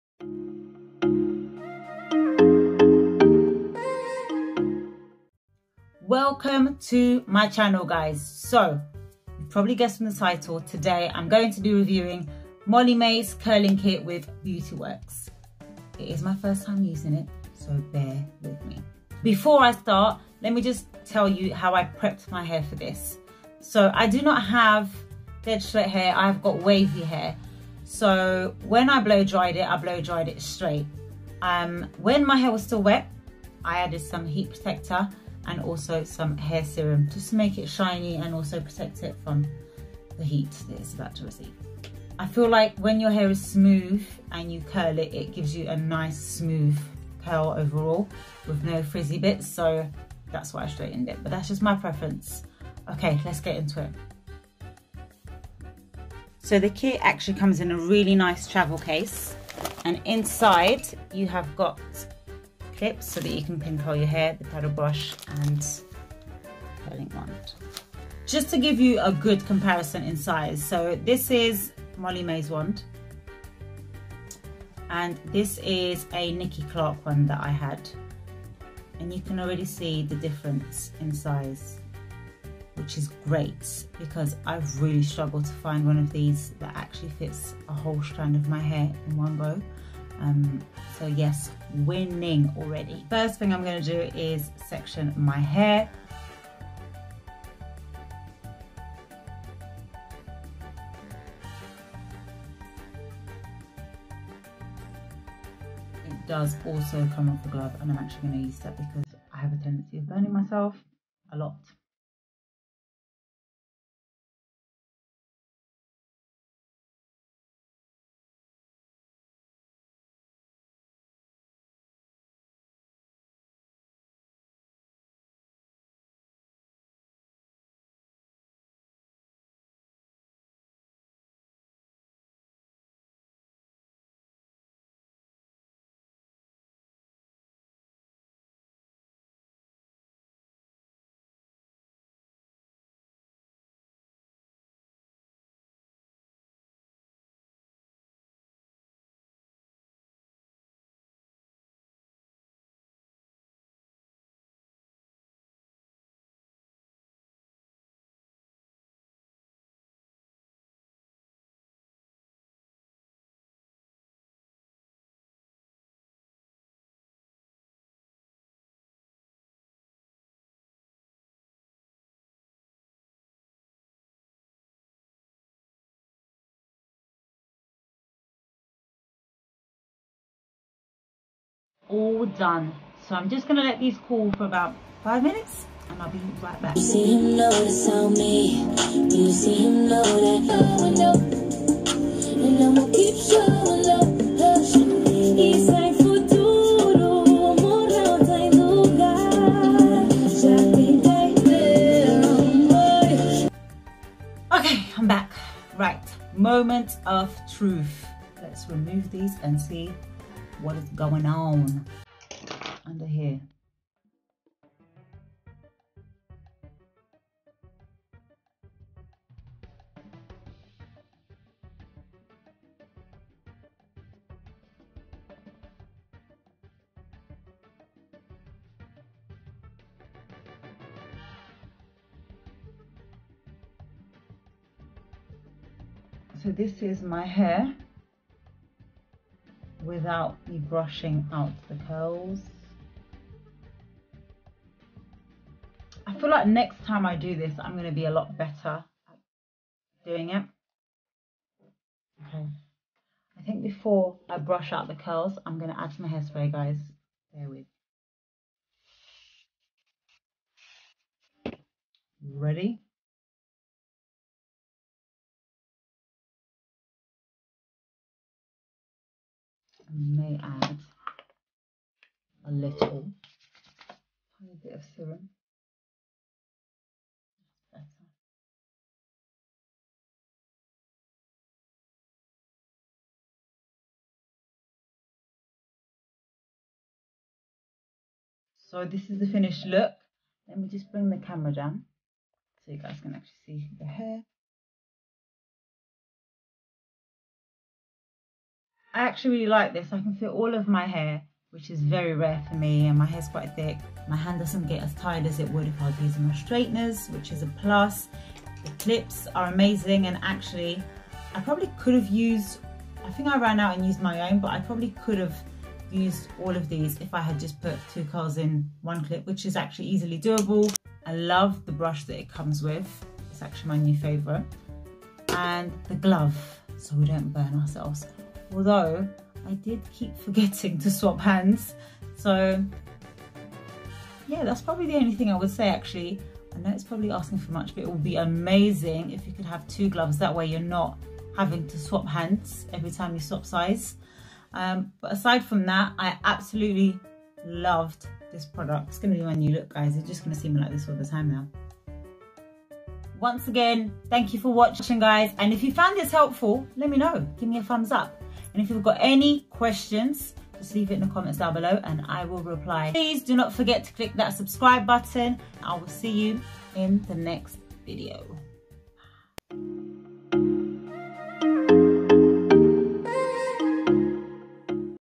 MOLLY MAE CURL KIT x BEAUTY WORKS
I purchased this as I always struggle to find a curling wand that will give volume curls but also one that is long enough to fit a full strand of hair...

PRODUCTS USED IN THIS VIDEO:

Molly-Mae Curling Kit

Tony & Guy Radiating Tropical Oil

Mark Hill Soft Touch Hairspray

GHD Heat Protecting Spray

There is also a quick update video on my Instagram @everydayw0man 😘